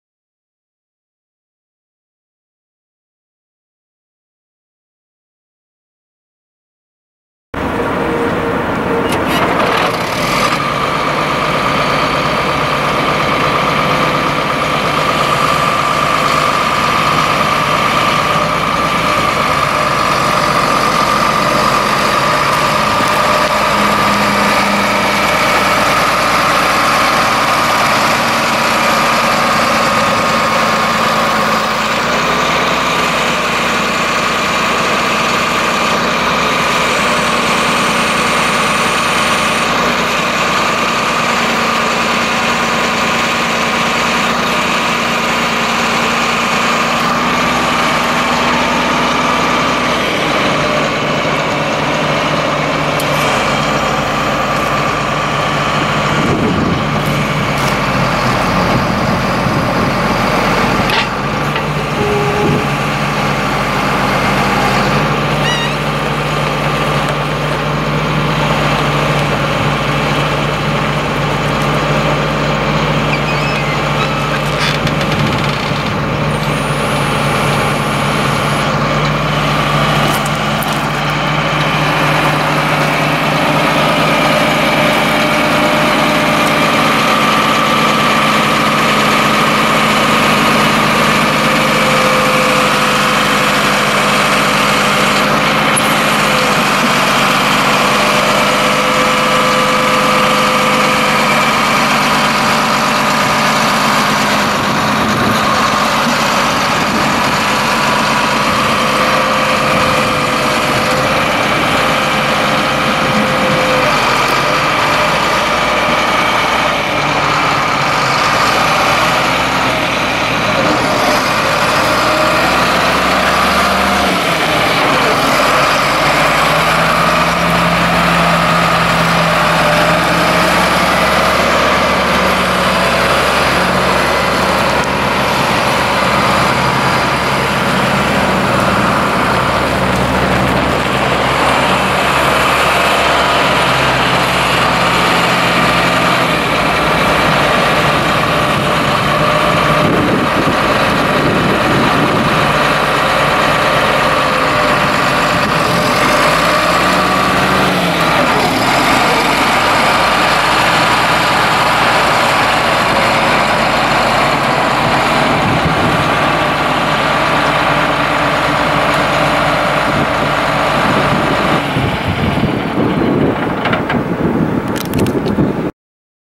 1989 John Deere 772BH motor grader for sale | sold at auction May 15, 2014
SOLD! Aggregate and Construction Equipment Auction

1989 John Deere 772BH motor grader
13,148 hours on meter
John Deere 6-466A six cylinder diesel engine
150 HP
Powershift transmission
8F-4R gears
All wheel drive
Heat
Front work lights
16&#039; mold board
Tip control
Side shift
Front mounted lift group
(7) control valves
17.5R25 tires
34,492 lbs. base operating weight
Former county machine
Serial DW772BH526143